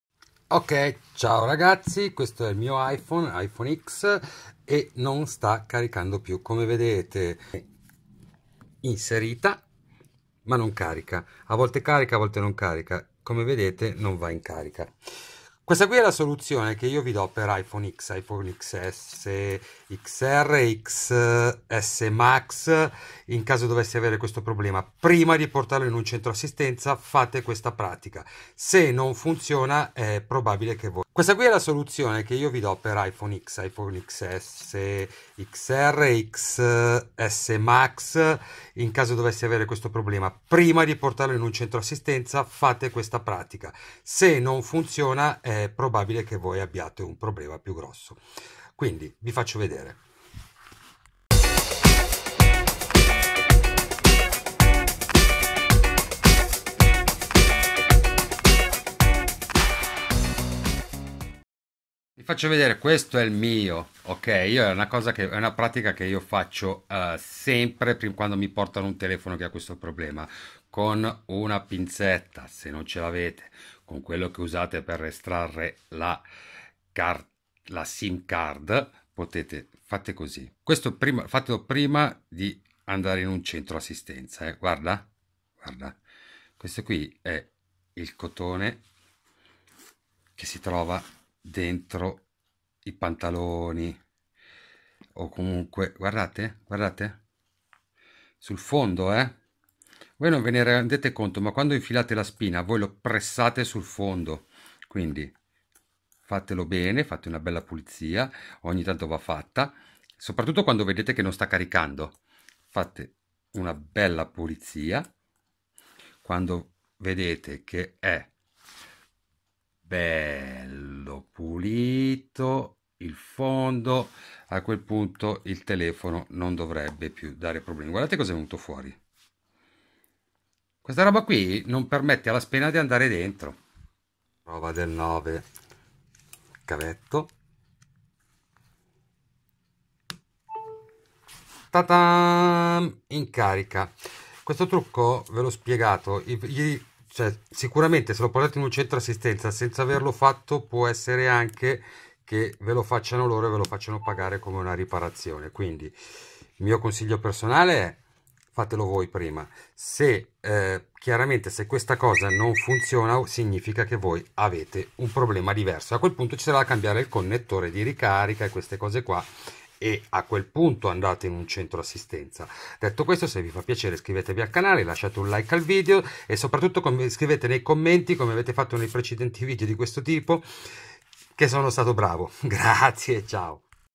iphone x xs xr xs max non carica? Ecco la soluzione prima di portarlo in assistenza
Hai l'iphone x xs xs max xr che non carica più? Ecco come risolvere in 5 minuti prima di portare il tuo iphone in assistenza o pensare di sostituire il connettore di ricarica

Pronto a riparare? Fatti trovare dai clienti della tua zona con gli annunci gratis su